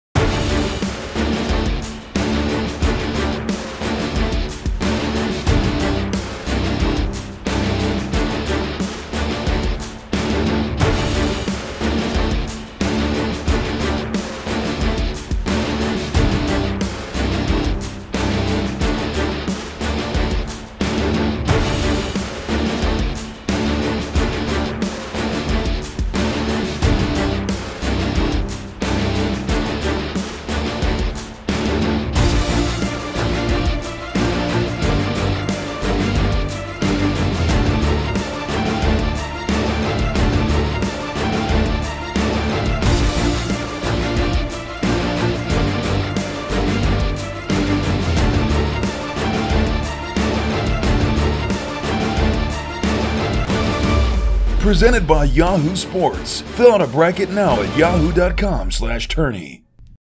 Butler Bulldogs NCAA Tournament Prediction | CampusInsiders
The Butler Bulldogs are heading back to the NCAA Tournament. To help fill out your bracket, Campus Insiders is your only source for "68 in 60 Powered by Yahoo." In 60 seconds, we'll break down Butler's strengths and weaknesses, X-factor, and how far the Bulldogs will go in March.